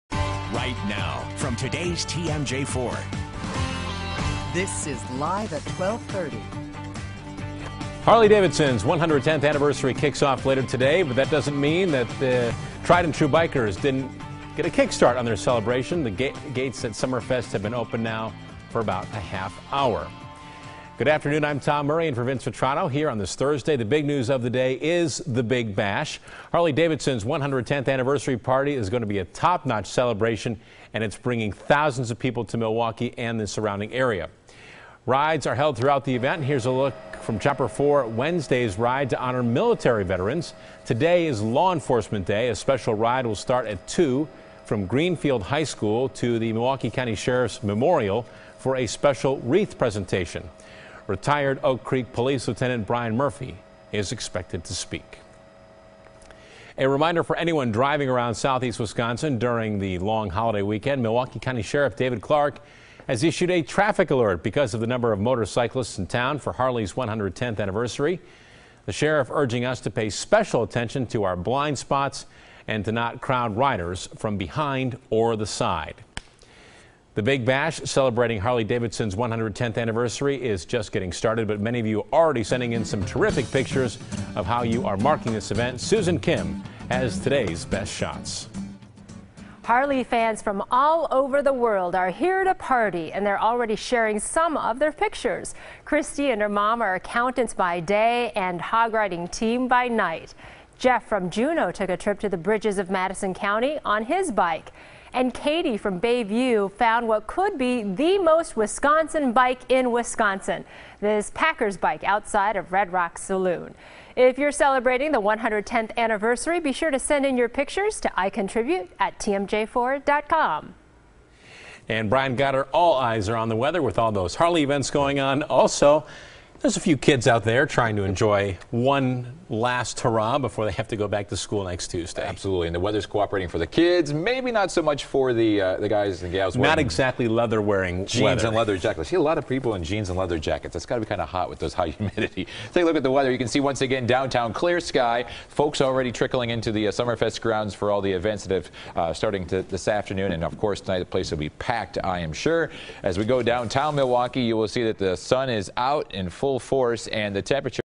Harley-Davidson kicks off 110th celebration
Harley riders made their way into town to celebrate the anniversary.